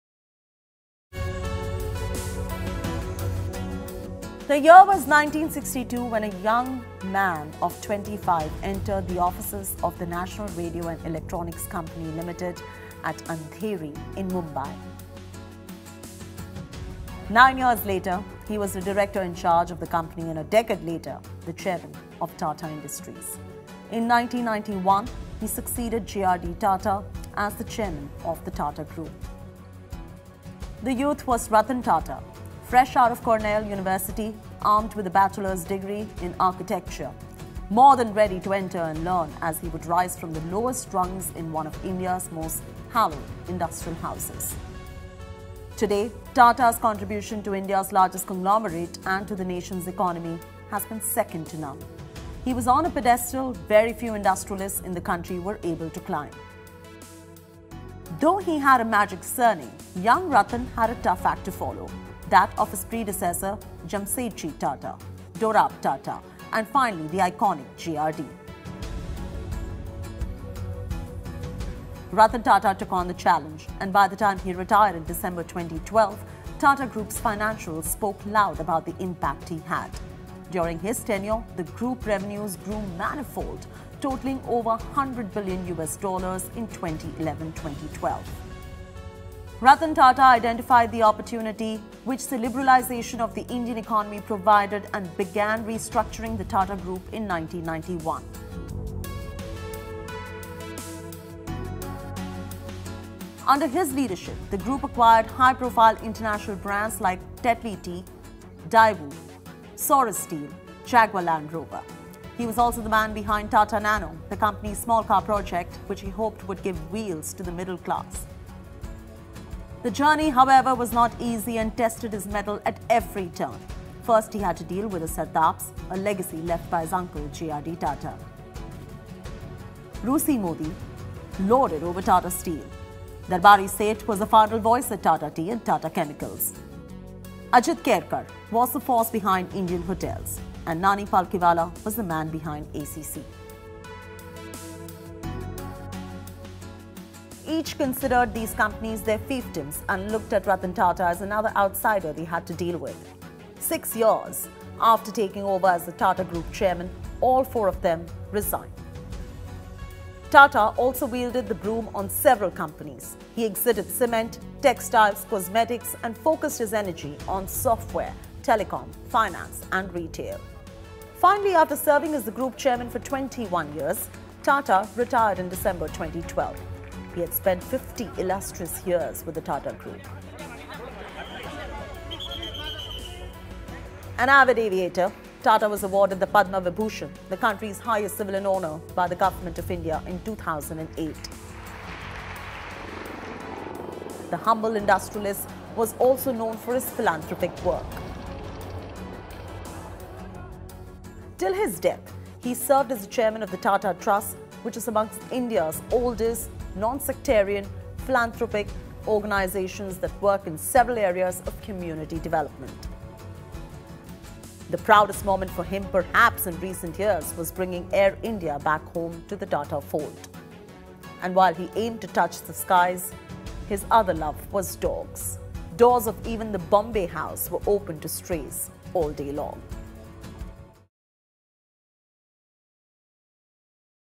Mr Ratan Naval Tata | Biography
Ratan Tata, the chairman emeritus of Tata Sons, hwas admitted to A Mumbai hospital on Monday. Hours later, Tata, in a statement, said he was undergoing routine medical investigations due to his age and related medical conditions. The veteran industrialist said there was no cause for concern regarding his health and he was undergoing check-ups for age-related medical conditions.

"I am currently undergoing medical check-ups due to my age-related medical conditions," he asserted. "There is no cause for concern. I remain in good spirits," he said, requesting the public and media to refrain from "spreading misinformation". Tata became chairman of the Tata Group, an autos-to-steel conglomerate, in 1991. He led the group, which was founded by his great-grandfather over a century ago, until 2012. In 1996, Tata founded Tata Teleservices, a telecommunications company, and took Tata Consultancy Services (TCS) public in 2004. After stepping down as chairman, Tata was honored with the title of chairman emeritus of Tata Sons, Tata Industries, Tata Motors, Tata Steel, and Tata Chemicals.

The Most Humble Richest Man On Earth Who Lives In Our Hearts Forever